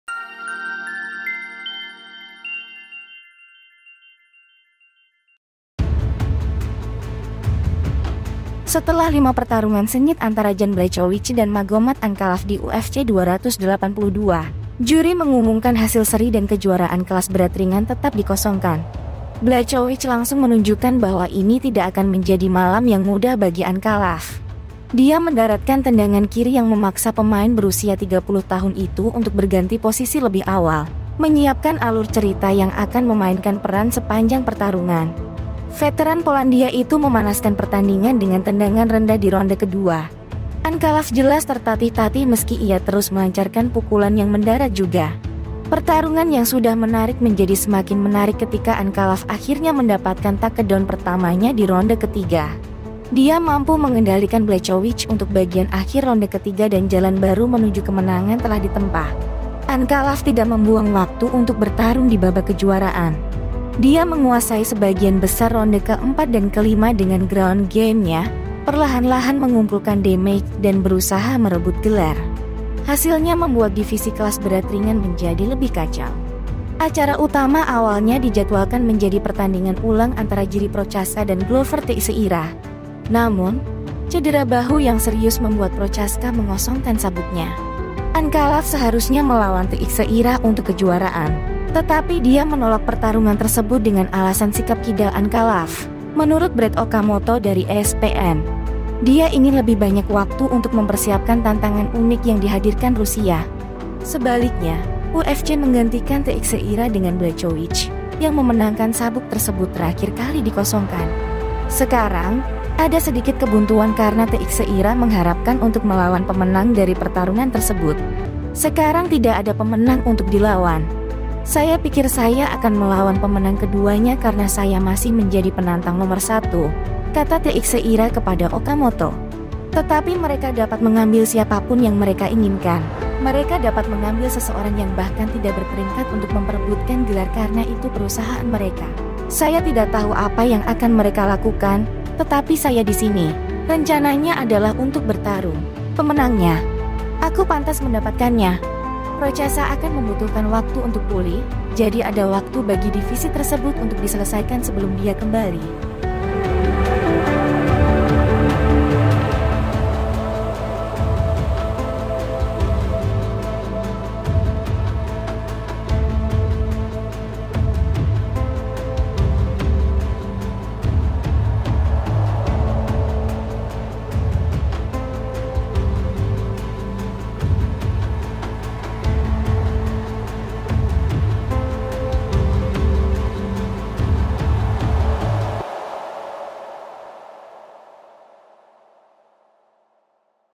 Jan Błachowicz vs Magomed Ankalaev Berakhir dengan Split Draw di UFC 282
Setelah lima pertarungan sengit antara Jan Błachowicz dan Magomed Ankalaev di UFC 282, juri mengumumkan hasil seri dan kejuaraan kelas berat ringan tetap dikosongkan.

Błachowicz langsung menunjukkan bahwa ini tidak akan menjadi malam yang mudah bagi Ankalaev.

Ankalaev jelas tertatih-tatih meski ia terus melancarkan pukulan yang mendarat juga. Pertarungan yang sudah menarik menjadi semakin menarik ketika Ankalaev akhirnya mendapatkan takedown pertamanya di ronde ketiga.

Dia mampu mengendalikan Błachowicz untuk bagian akhir ronde ketiga dan jalan baru menuju kemenangan telah ditempa. Ankalaev tidak membuang waktu untuk bertarung di babak kejuaraan.

Dia menguasai sebagian besar ronde keempat dan kelima dengan ground game-nya, perlahan-lahan mengumpulkan damage dan berusaha merebut gelar. Hasilnya membuat divisi kelas berat ringan menjadi lebih kacau.